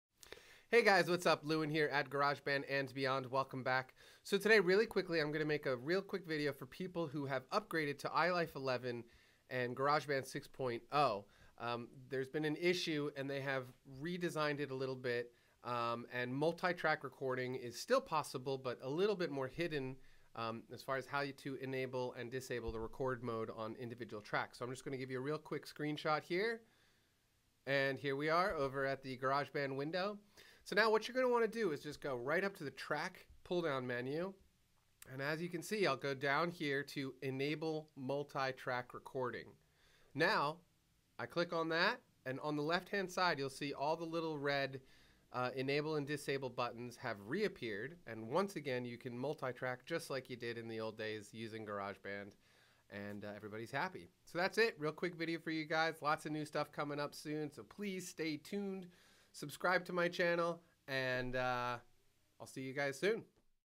How to Multi Track in Garageband 11, iLife 11
There have been some changes to Garageband inside of iLife 11. In this video I show you how to Multi Track record just like in the older versions of GB.